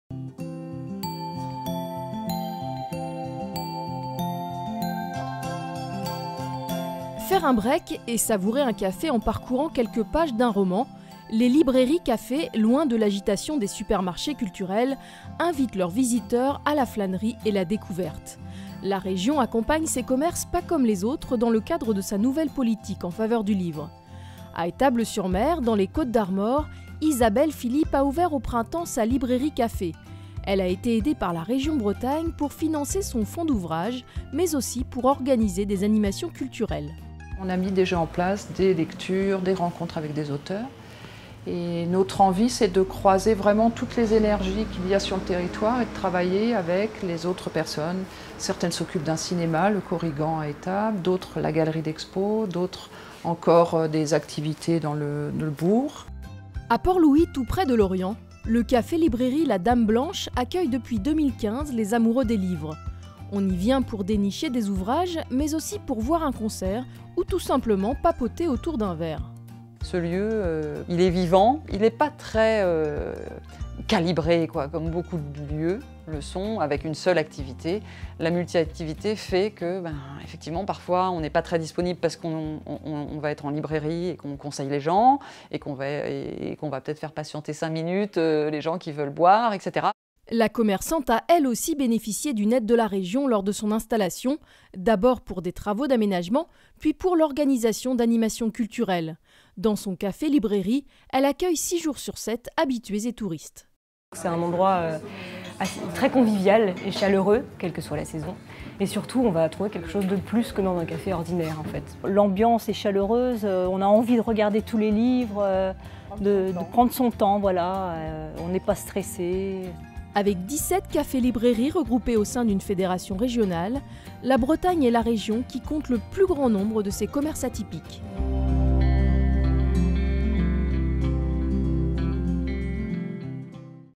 Bretagne: La Region soutient ses cafés librairies
La Région Bretagne soutient ses cafés-librairies, réseau culturel de proximité pour tous. Vidéo diffusée dans l'atelier 9 "la culture près de chez moi" du Congrès 2019 des Régions, le 30 septembre à Bordeaux-Nouvelle-Aquitaine.